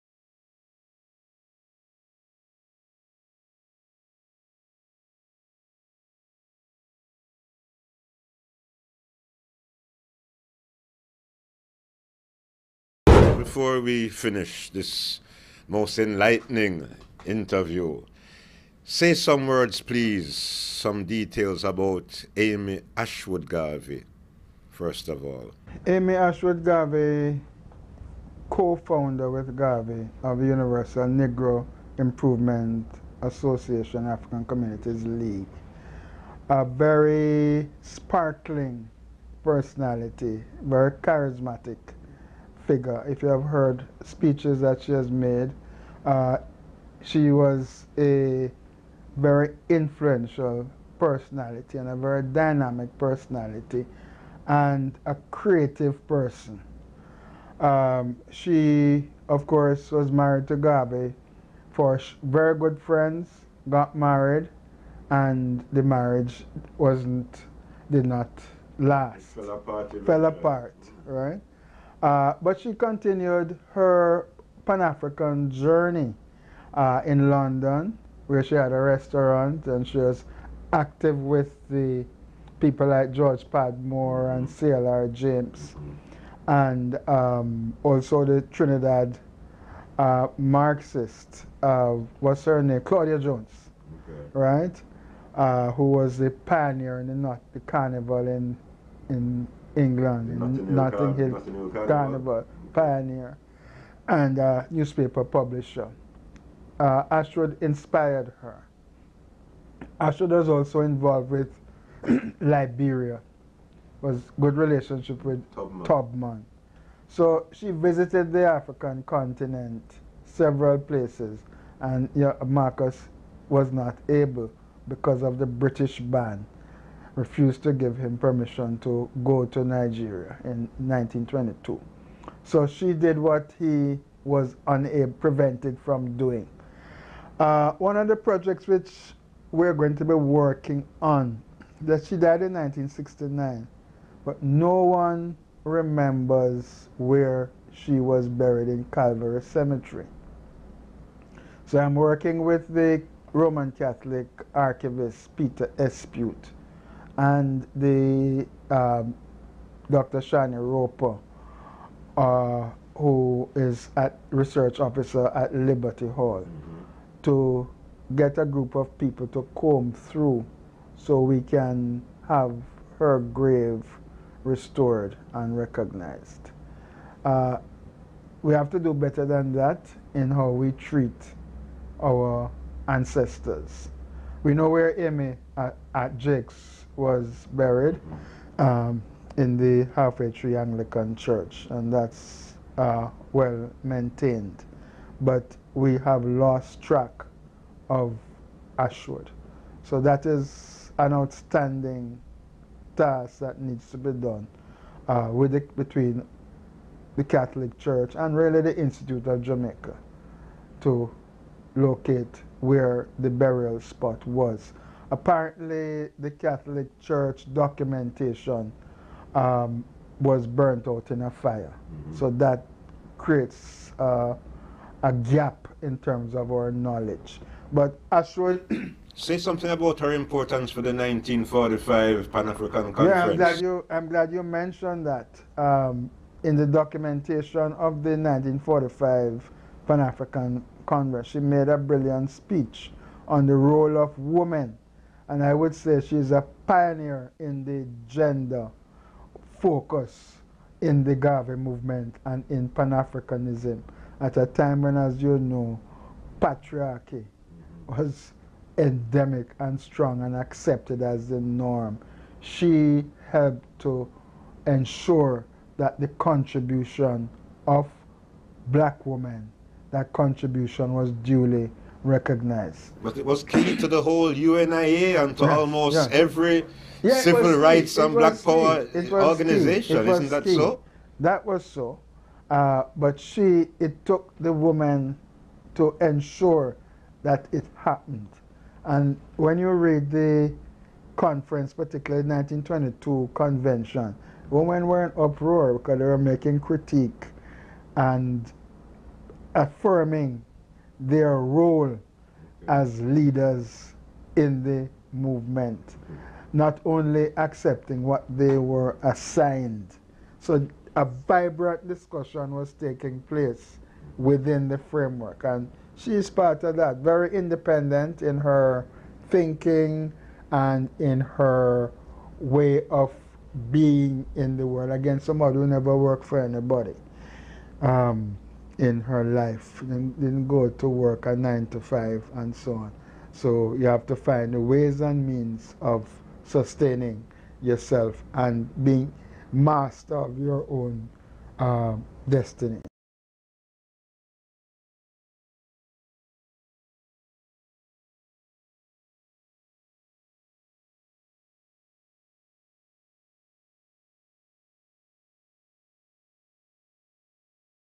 Rupert Lewis Speaks Garvey Part 4 of 5 Amy Ashwood
PROFESSOR RUPERT LEWIS SPEAKS GARVEY AND GARVEYISM - Part 4 of 5:
Garvey Scholar Professor Rupert Lewis speaks with journalist I. Jabulani Tafari about UNIA co-founder Amy Ashwood Garvey and her contribution to the Garvey Movement and the African liberation struggle. Professor Lewis further speaks about Amy Ashwood’s role in the 5th Pan African Congress in Manchester, England in 1945, and speaks about the ongoing project to locate her burial site in Calvary cemetery in Kingston.